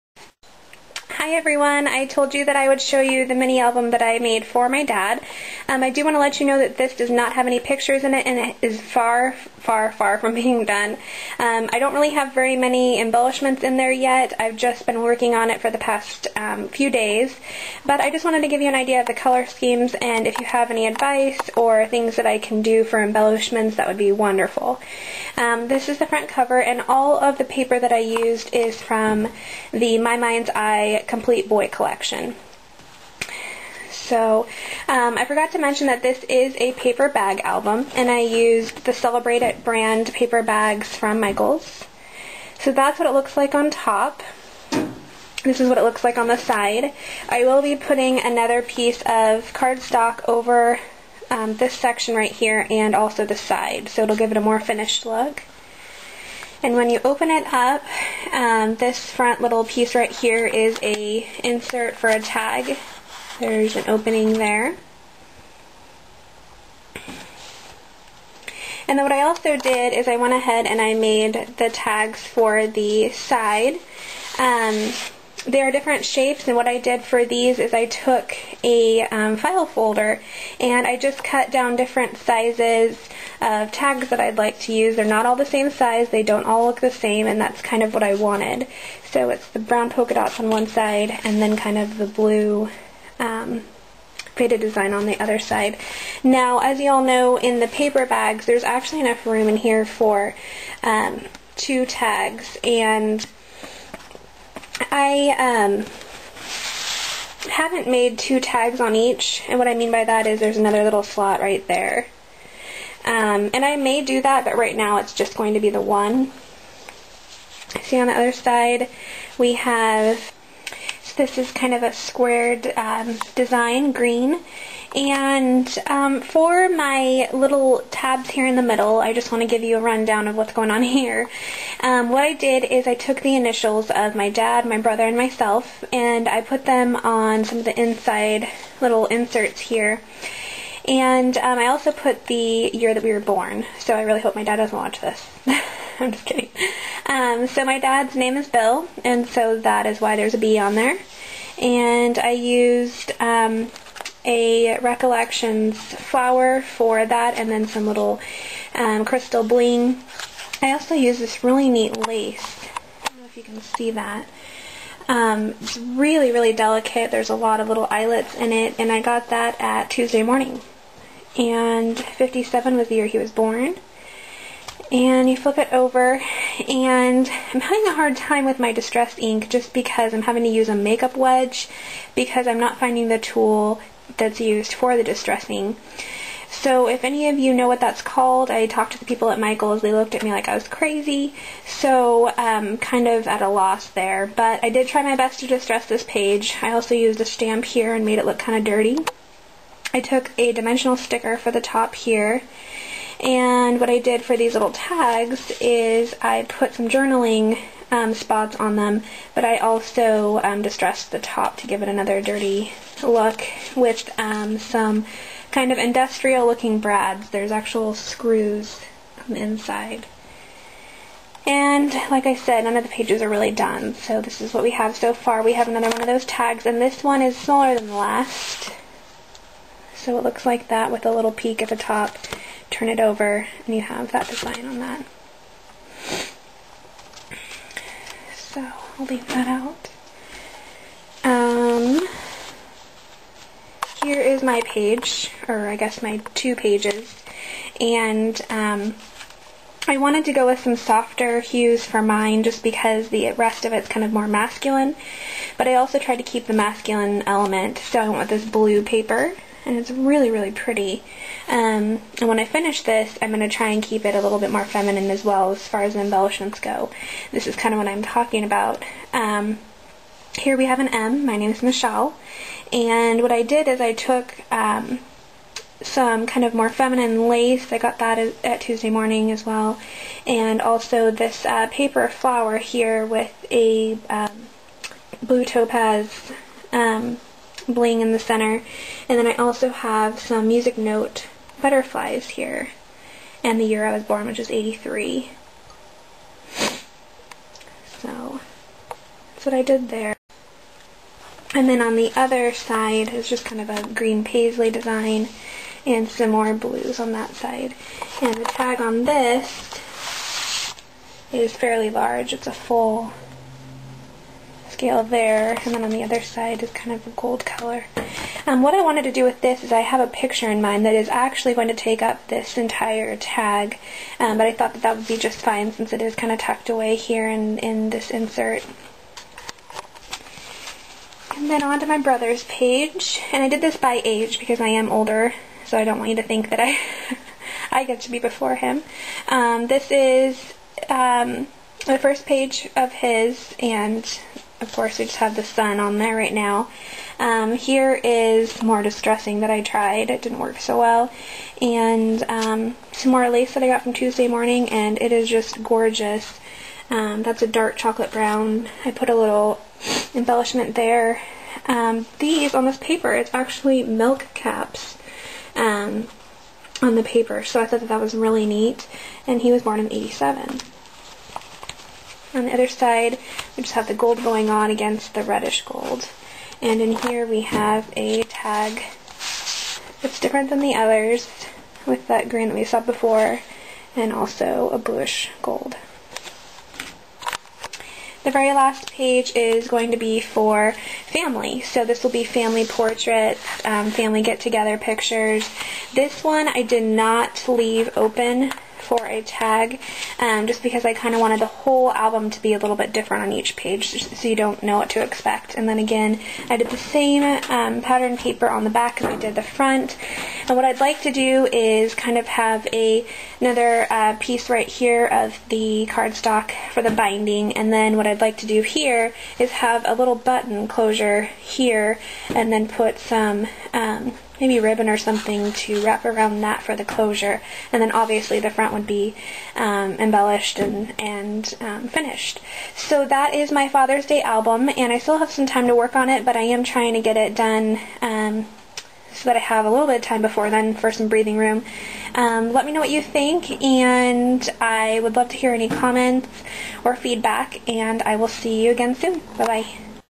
Fathers Day Paper Bag Mini Scrapbook Album
Here's a look at my progress into my Fathers Day Mini.